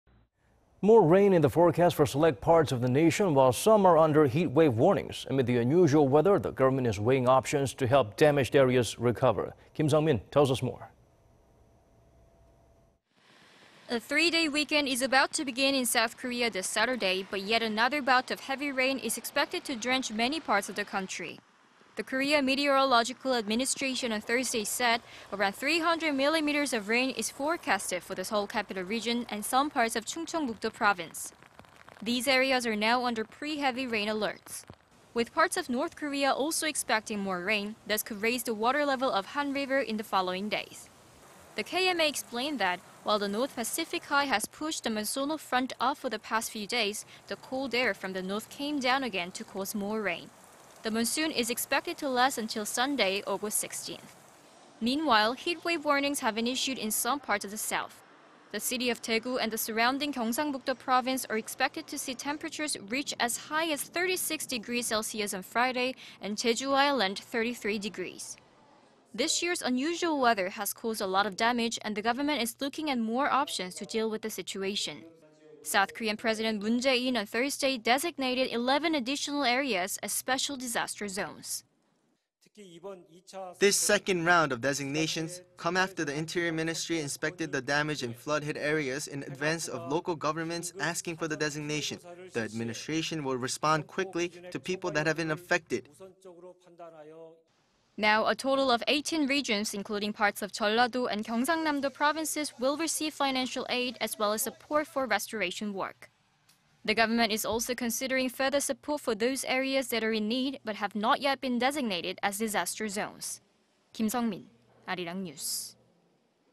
S. Korea to see either heavy rain or heatwave during long weekend
험난한 연휴 날씨…최대 300㎜ 폭우, 아니면 최고 36도 폭염

More rain in the forecast for select parts of the nation while some are under heatwave warnings.
Amid the unusual weather, the government is weighing options to help damaged areas recover.
Kim Sung-min tells us more.
A three-day weekend is about to begin in South Korea this Saturday but, yet another bout of heavy rain is expected to drench many parts of the country.
The Korea Meteorological Administration on Thursday said around 300 millimeters of rain is forecasted for the Seoul capital region and some parts of Chungcheongbuk-do Province.
These areas are now under pre-heavy rain alerts.
With parts of North Korea also expecting more rain, this could raise the water level of Han River in the following days.
The KMA explained that, while the North Pacific High has pushed the monsoonal front up for the past few days, the cold air from the North came down again to cause more rain. The monsoon is expected to last until Sunday August 16th.
Meanwhile, heatwave warnings have been issued in some parts of the south.
The city of Daegu and the surrounding Gyeongsangbuk-do Province are expected to see temperatures reach as high as 36 degrees Celsius on Friday...and Jeju Island 33 degrees.
This year's unusual weather has caused a lot of damage, and the government is looking at more options to deal with the situation.
South Korean President Moon Jae-in on Thursday designated 11 additional areas as special disaster zones.
"This second round of designations come after the interior ministry inspected the damage in flood-hit areas in advance of local governments asking for the designation. The administration will respond quickly to people that have been affected."
Now a total of 18 regions, including parts of Jeolla-do and Gyeongsangnam-do provinces will receive financial aid as well as support for restoration work.
The government is also considering further support for those areas that are in need but have not yet been designated as disaster zones.
Kim Sung-min, Arirang News